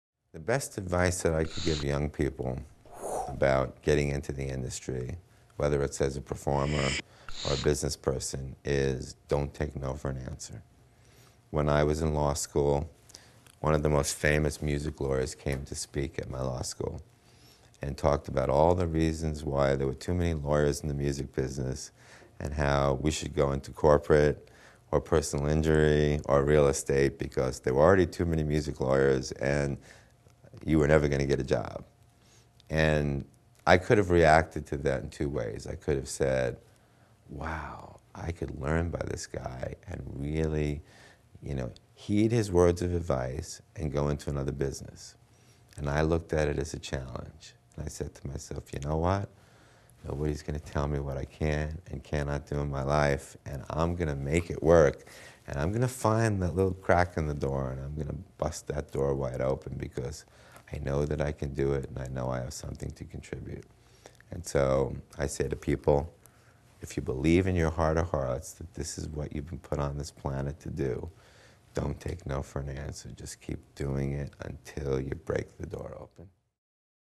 David Helfant On Advice On Getting Into the Music Industry
- David Helfant is the Vice President of Business and Legal Affairs at the music division of Paramount Pictures. In this segment he gives advice about getting into the music industry. His main piece of adivcie to never take no for an answer and be persistent about your career plans.